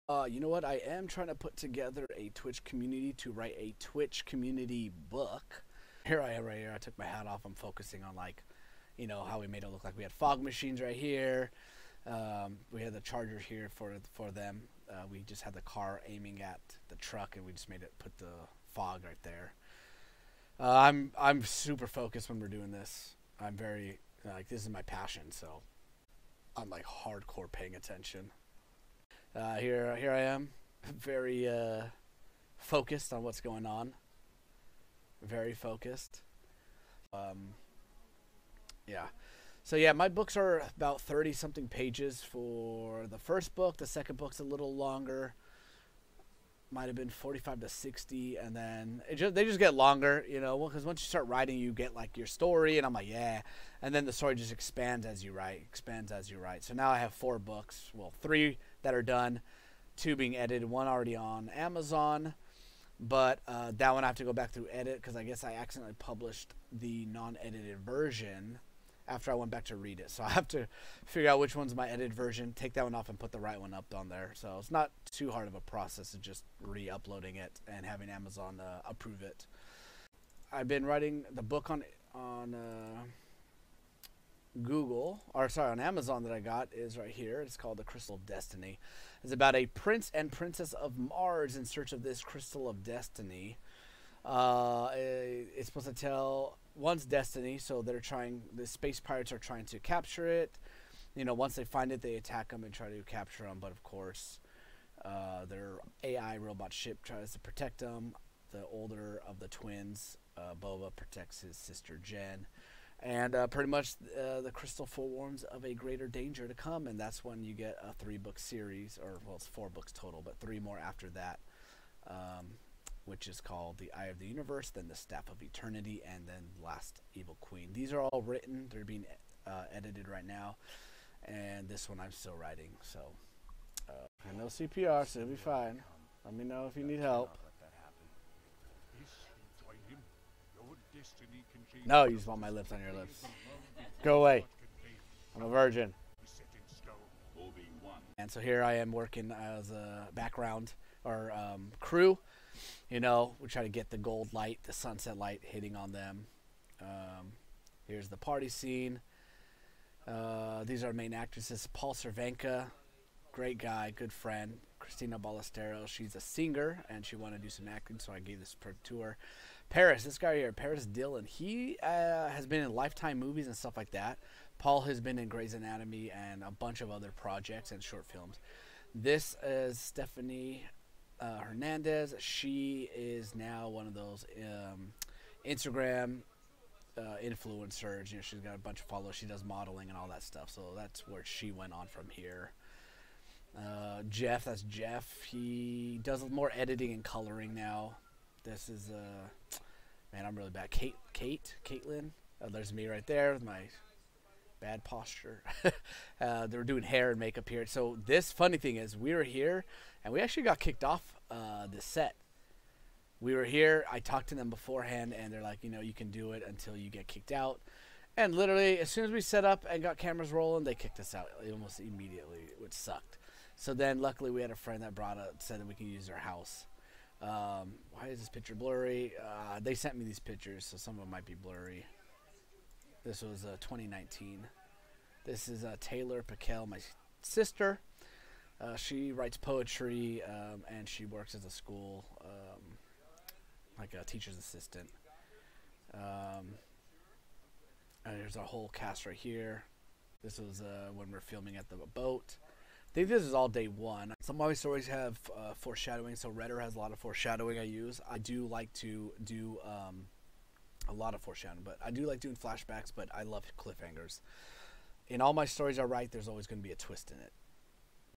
Writing Tips for Beginners | Creative Writing Video
FREE PDF WRITING WORKSHEET

In this video, I'll be going over a few writing tips from my live clips.

Welcome to my Writing Montage Clips.

Justin Van Der Mark
Creative Writing Degree from UC Riverside

💖My Kids Book:

🥤 ► Since you made it this far in the description, comment 'your favorite book'!